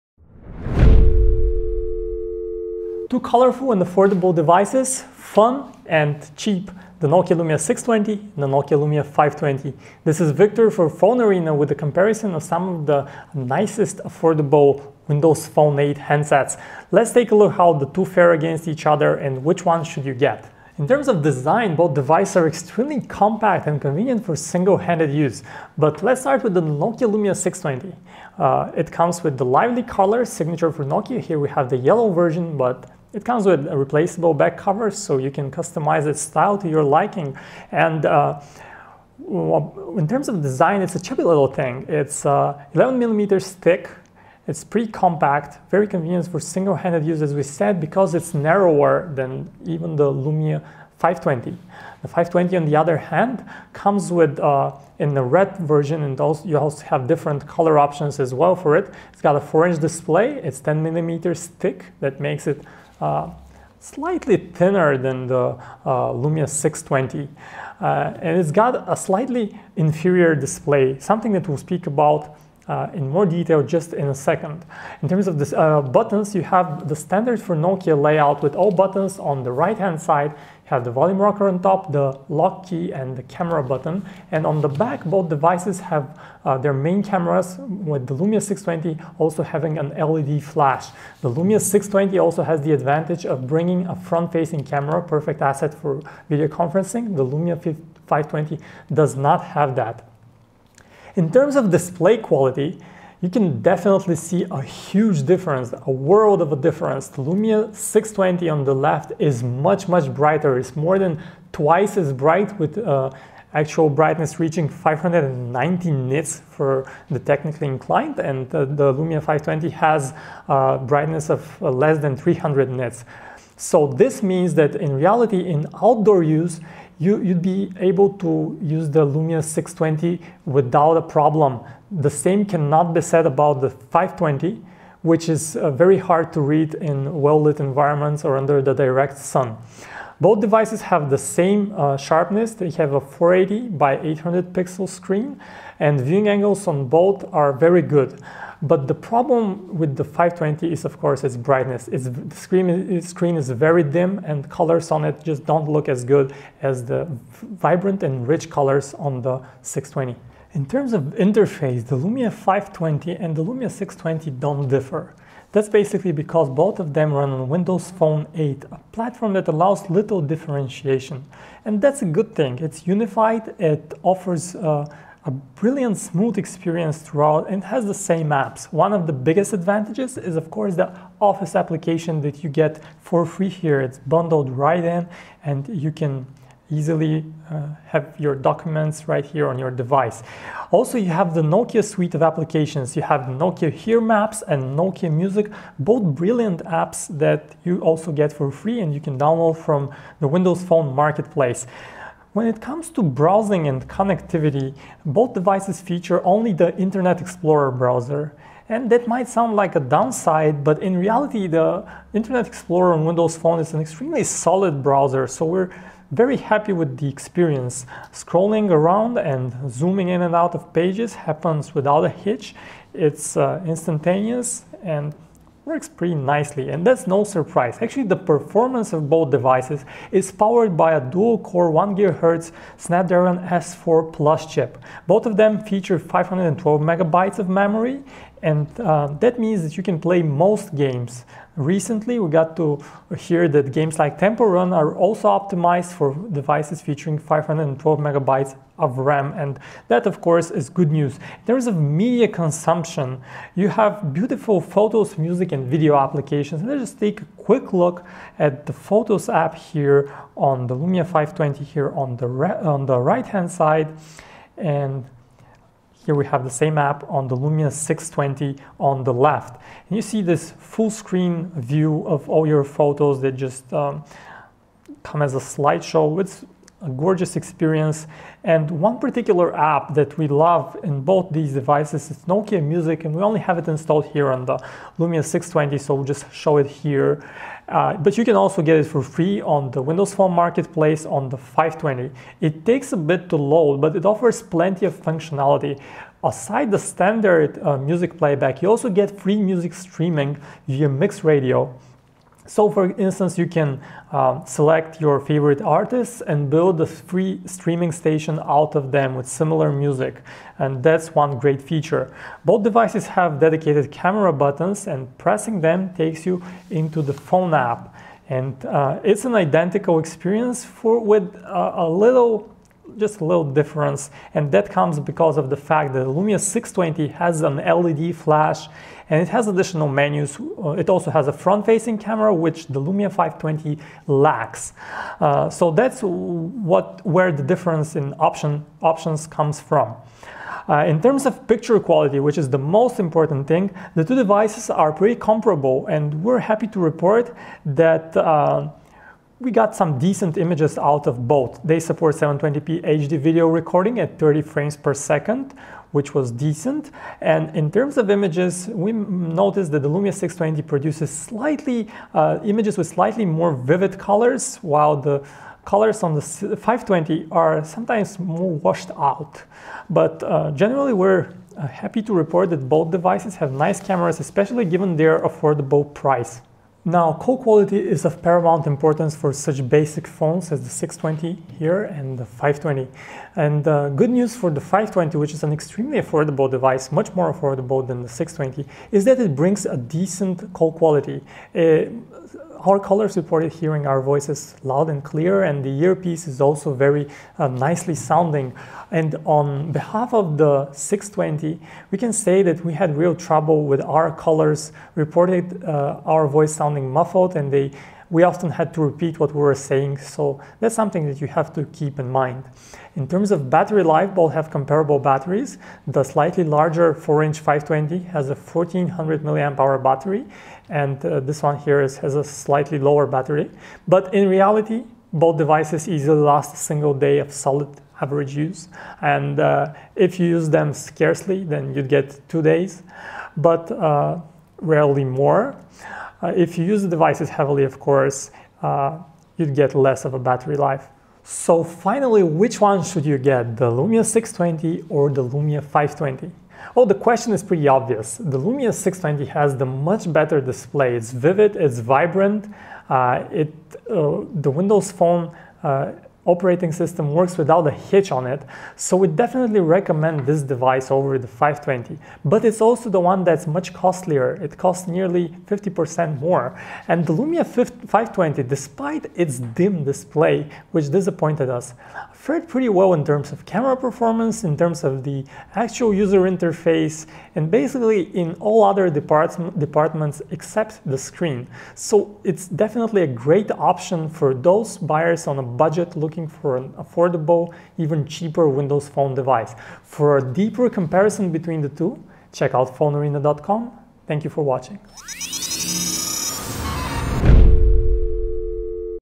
Nokia Lumia 520 vs Nokia Lumia 620
It's time to compare two affordable and cute compact Windows Phone 8 handsets. Both are colorful and fun on the outside, but could there still be a sea of difference inside?...